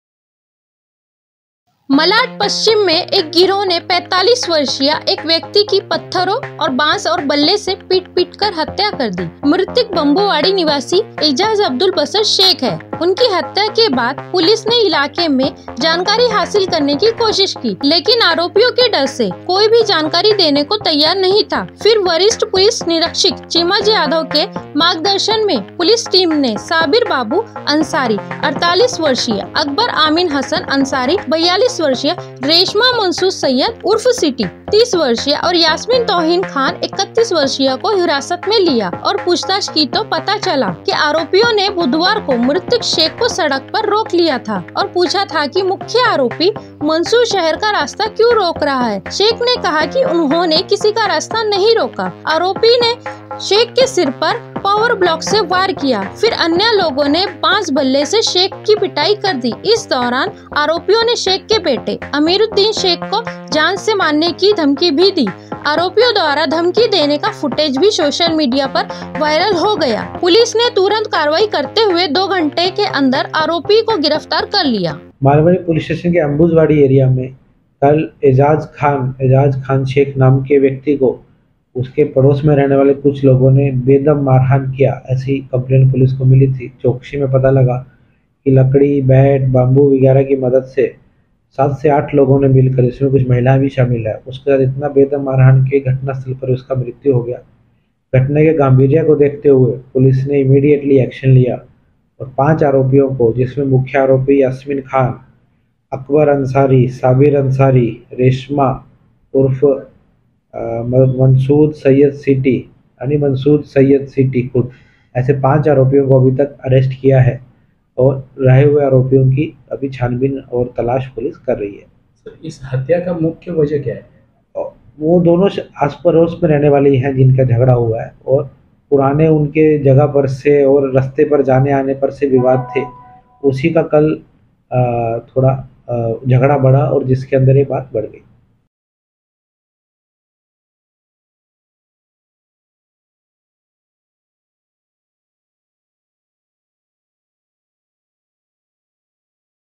45 वर्षीय Ejaz की पत्थरों, बांस और बल्ले से पीट-पीट कर हत्या || Aapka Prahar Times
45 वर्षीय Ejaz की पत्थरों, बांस और बल्ले से पीट-पीट कर हत्या II Aapka Prahar Times